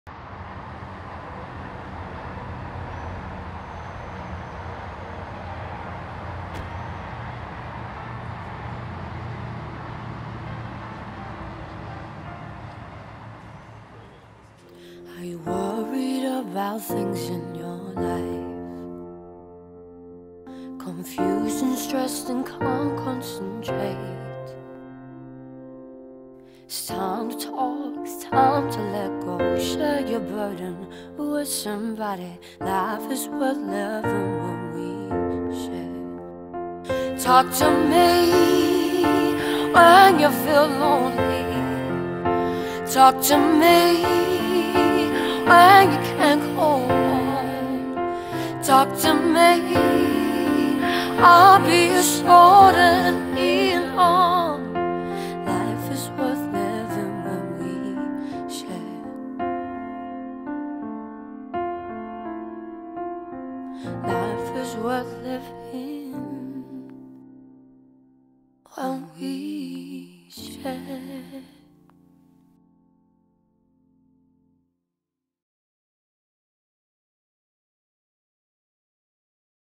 Talk2Me - One life lost is one too many...
Today marks World Suicide Prevention Day. Nearly 6,000 people die by suicide each year. The Zero Suicide Alliance are urging everybody to take the free training, which could help save a life.